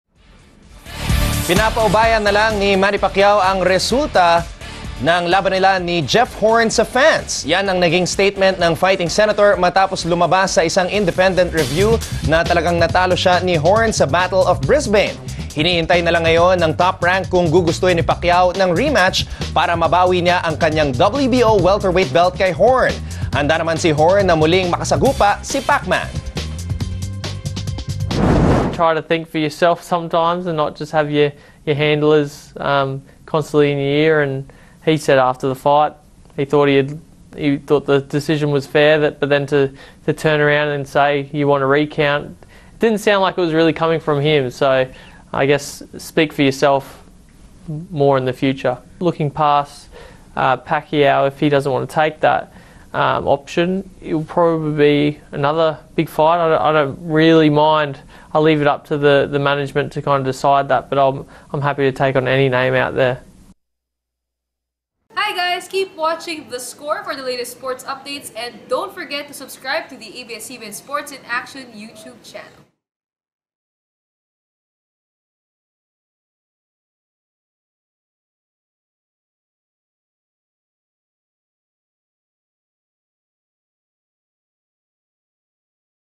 The Score: Pacquiao-Horn rematch
Australian boxer Jeff Horn says he is ready for a rematch against Manny Pacquiao.